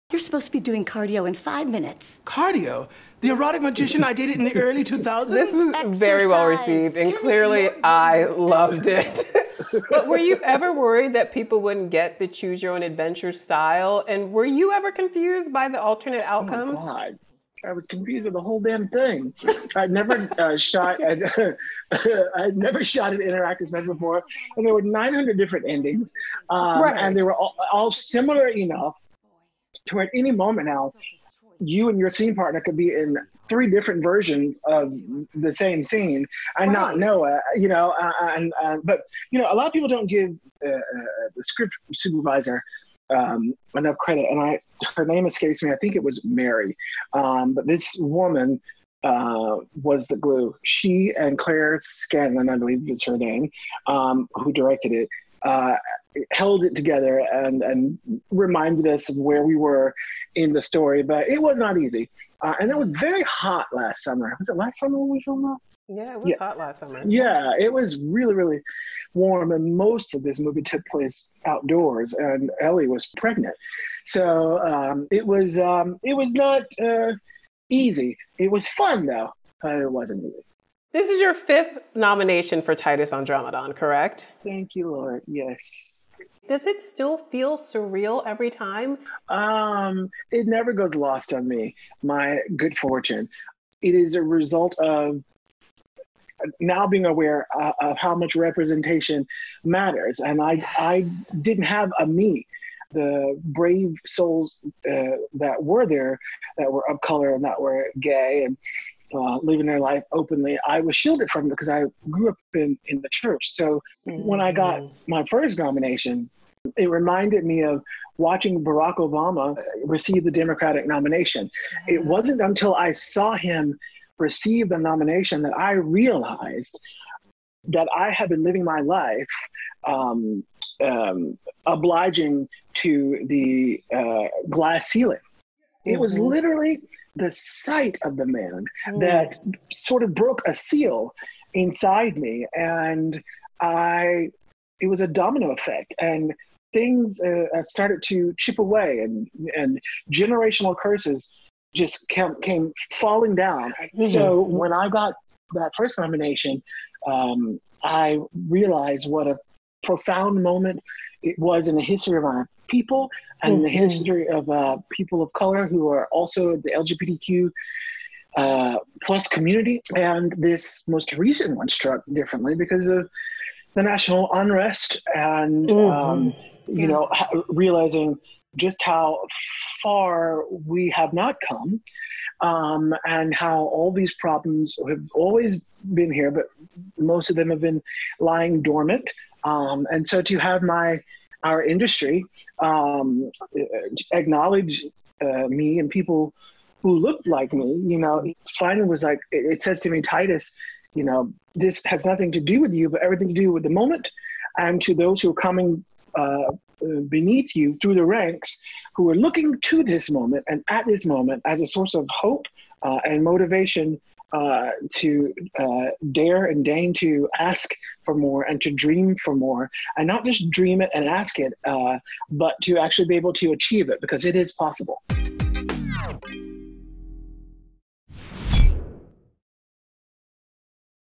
Tituss Burgess Talks 'Unbreakable Kimmy Schmidt' & Emmy Nomination | PeopleTV | Entertainment Weekly
Tituss Burgess looks back on the complexities of shooting the interactive 'Unbreakable Kimmy Schmidt' special and how it got made, and discusses the importance of representation in the media and what his Emmy nomination represents!